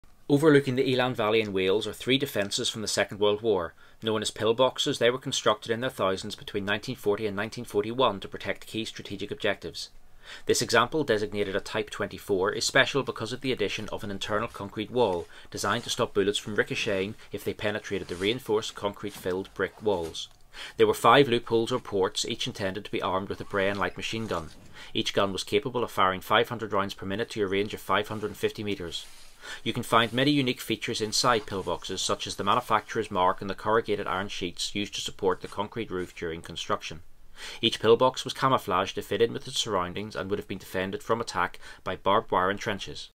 Second World War pillbox overlooking the Elan Valley, Wales
Overlooking the Elan Valley in Wales are three defences from the second world war. Known as pillboxes, they were constructed in their thousands between 1940 and 1941 to protect key strategic objectives.

This example, designated a type 24, is special because of the addition of an internal concrete wall designed to stop bullets from ricocheting around the inside if they penetrated the reinforced concrete filled brick walls.

There were 5 loopholes or ports, each intended to be armed with a Bren light machine gun. Each gun was capable of firing 500 rounds per minute to a range of 550 meters.

You can find many unique features inside many pillboxes, such as the manufacturers mark on the corrugated iron sheets used to support the concrete roof during construction.

Each pillbox was camoflagued to fit in with its surroundings and would have been defended from attack by barbed wire and trenches.